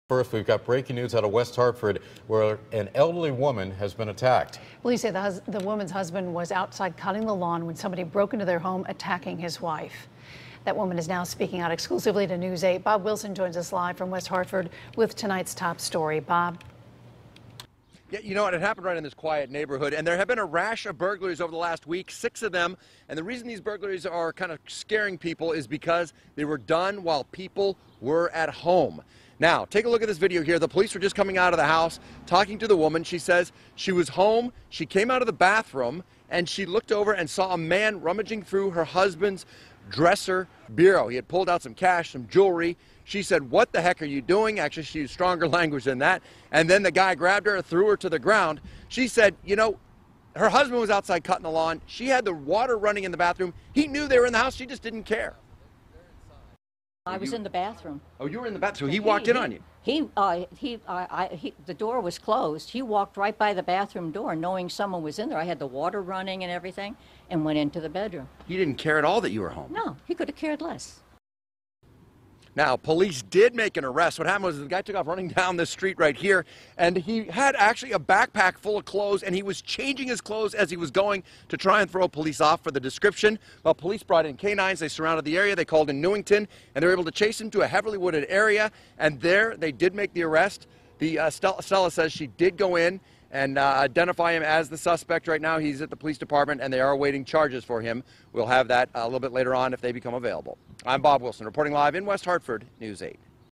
Elderly woman attacked in home during break in
An elderly woman was attacked in her West Hartford home after someone broke in this afternoon.